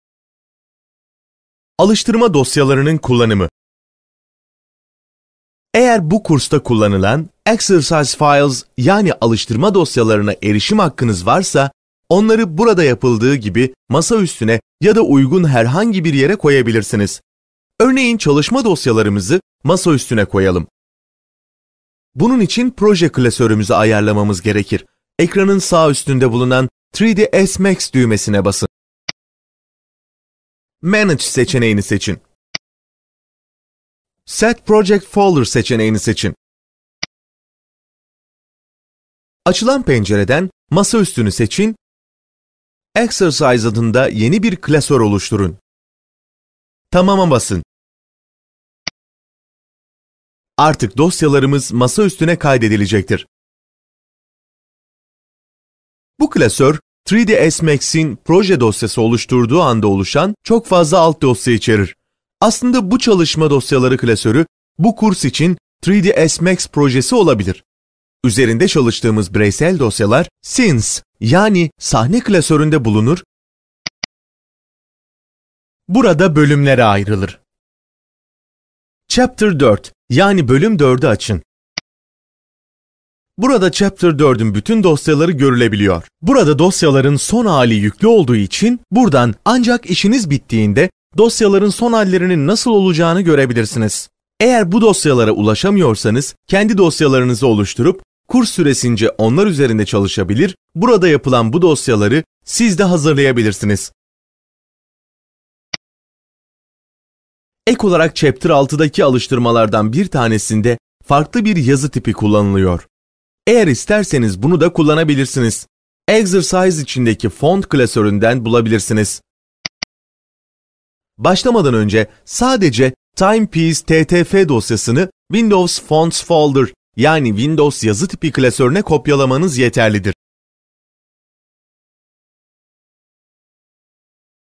Autodesk 3ds Max - 1. Giriş - 4. Alıştırma Dosyalarının Kullanımı
Çizgi TAGEM e-Kampüs Eğitim Seti 3ds Max Temel Eğitimi DVD'sinden alınarak yayınlanmıştır.
Bu eğitim filmi tamamen veya kısmen üçüncü kişiler tarafından satılamaz.
Bu içeriklerin eğitim amacıyla kullanımı, paylaşımı veya dağıtımı serbesttir.